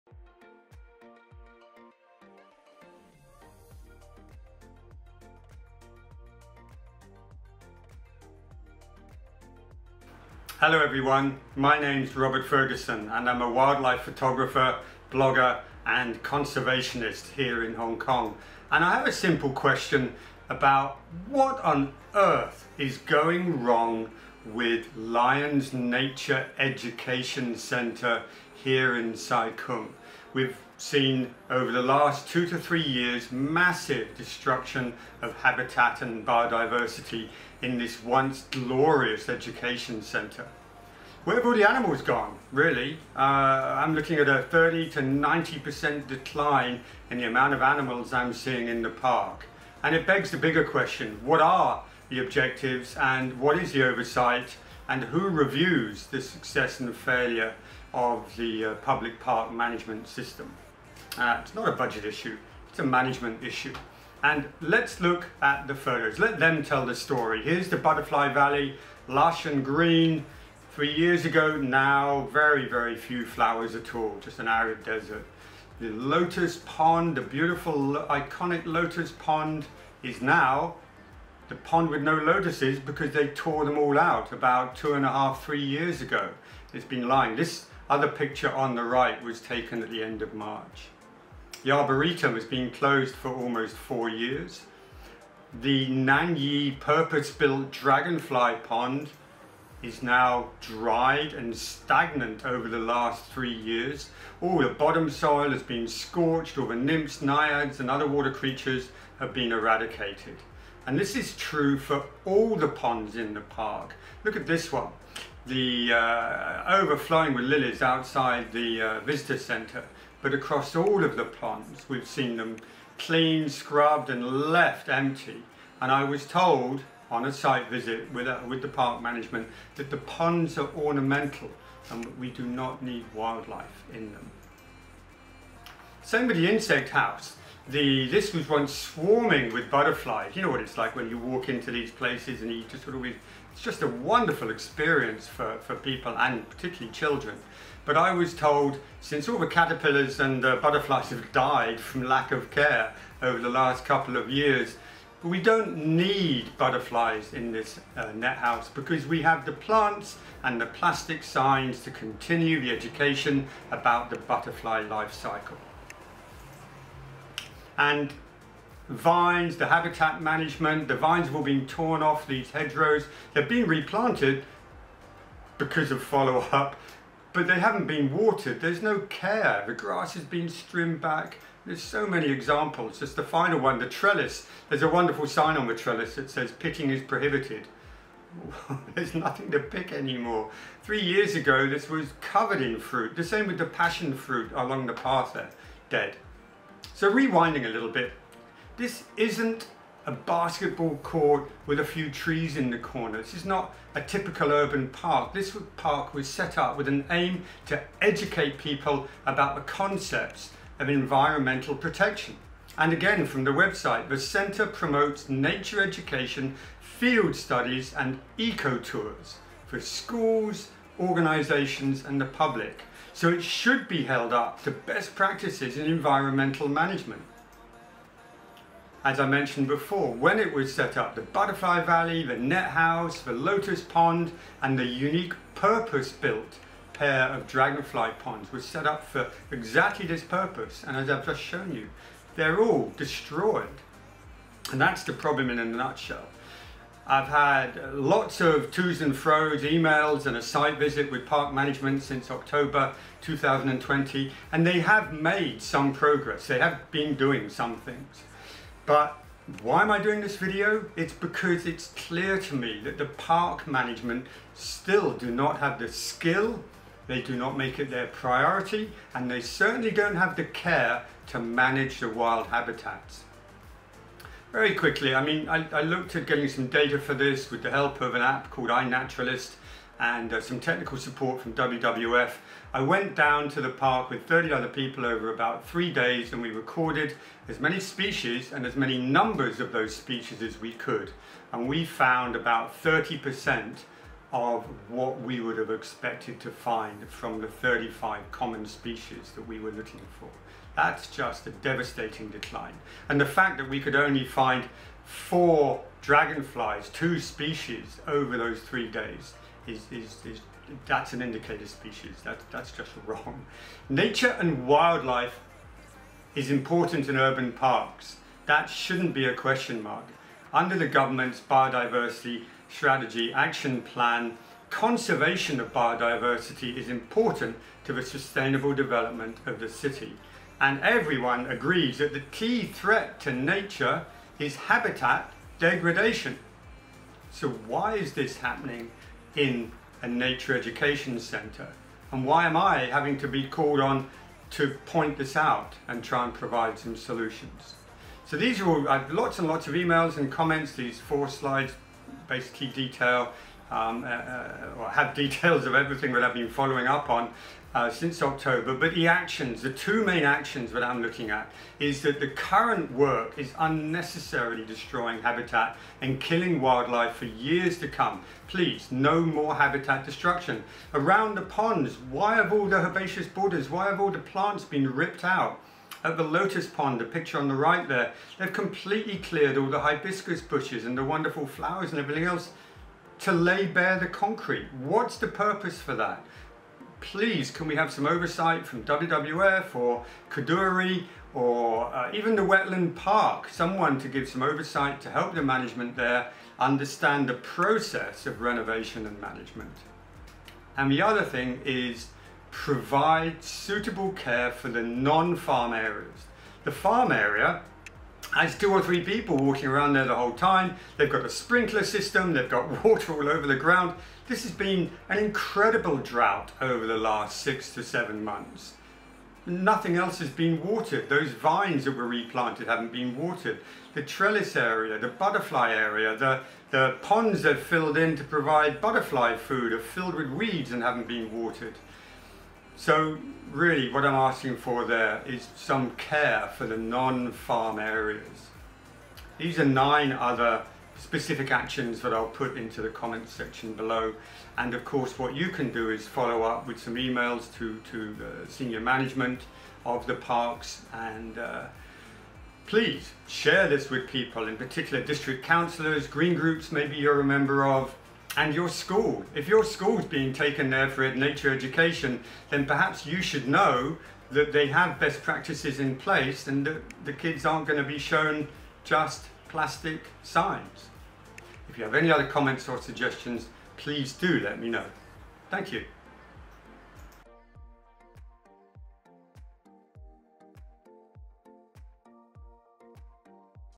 Lions Nature Education Centre April 2021. Serious habitat destruction & biodiversity concerns.
The 9 key actions for proper park management:
1. Provide suitable planning and expert supervision of any contractors contractors involved in habitat management and “cleaning”.
2. Provide suitable care for the non-farm areas: such as watering.
3. Restore the ponds with a suitable wildlife environment: bottom soil; lilies, and other plants, and borders.
 4. Restore the butterfly valley with suitable grasses, flowers, and shrubs.
5. Replant the lotus and lilies in the Lotus pond, and cover the bare concrete with hibiscus shrubs, like before.
6. Hedgerows: Stop replacing these with concrete posts. Stop the pulling off of vines and other worthwhile habitats from the fences. Let the vines grow up the fences.
7. Get rid of most - if not all - of the plastic fencing that is such an eyesore, and not part of any nature education I know of. Close the gates and keep the boars out.
8. Keep the “net-house” and restore it to its former glory. Re-instate the Birdwing Butterfly plant, and let this endangered species thrive again, as a model for the park.
9. Let the grasses grown in suitable “wild areas”, like in the fragrance garden, or on the slopes in the park.

If you would like to help follow up:
(Sample e-mail) Dear sir/madam:
I would like to voice my concern over the current management practices in the park, and I would like you to provide more care for the wild habitats.

It has been a challenge to find out, but I believe that these are the persons mainly responsible for the park management at the moment:

Let your district councillor know you care
Share this with everyone you know and who cares about our environment.  Get your school involved. Lobby any green or advocacy groups that you are part of. Take part in the forthcoming bio-blitz which Wildcreatures will be organising at LNEC.